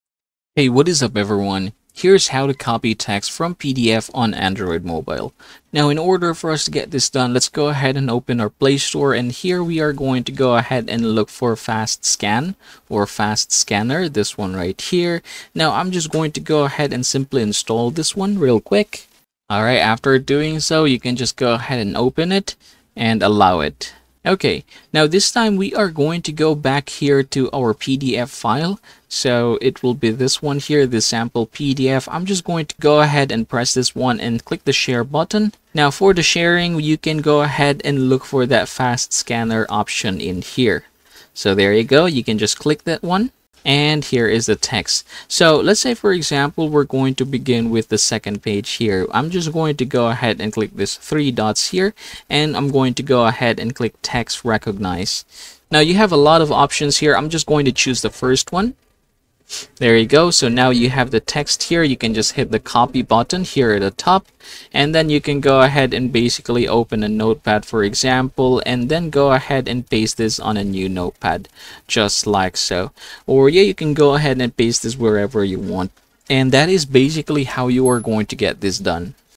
How To Copy Text From PDF On Android Mobile
Are you tired of manually typing out text from PDFs on your Android device? Do you wish there was a quicker and more efficient way to extract the information you need? Look no further! In this tutorial, we will show you how to easily copy text from PDFs on your Android mobile.